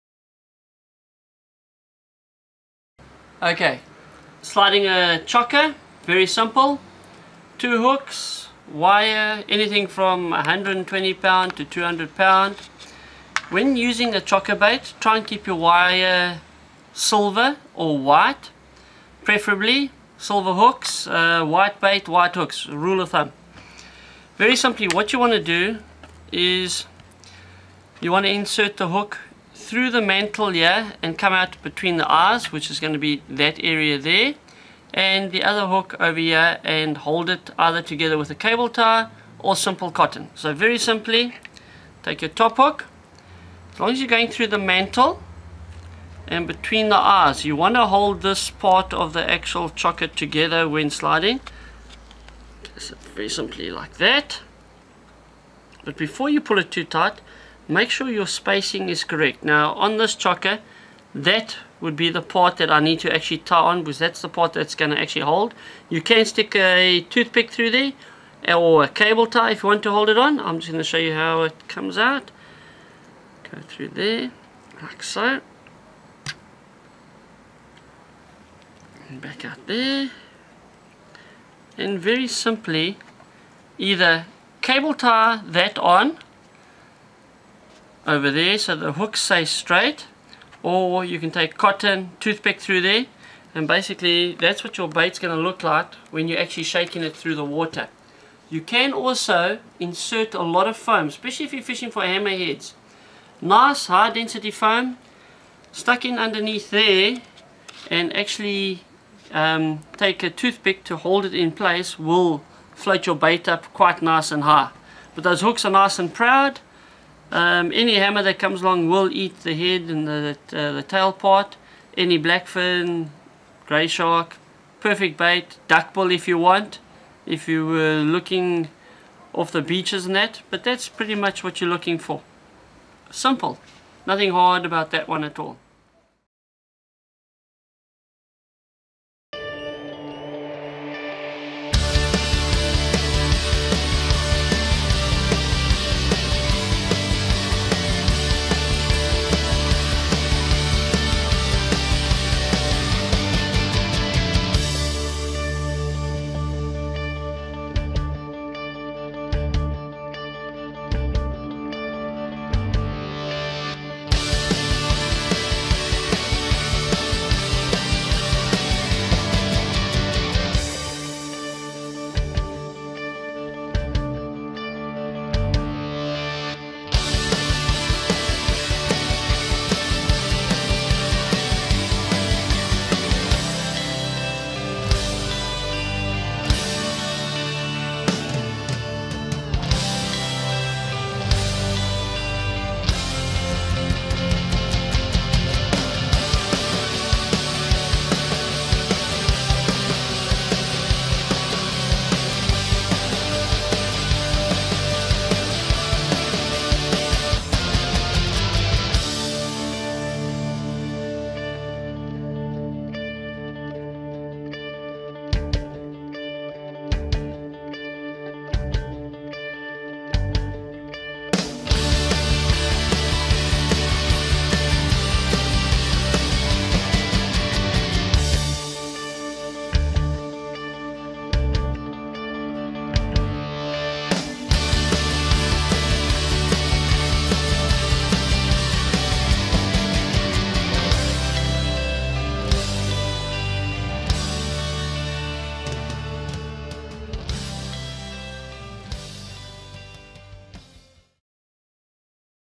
Sliding a Whole Chokka/Squid - Droning
Whether used for rock and surf sliding or dropped out with a drone, using a whole squid bait is bound to give you a great day out fishing.
Here we show you how to rig it correctly!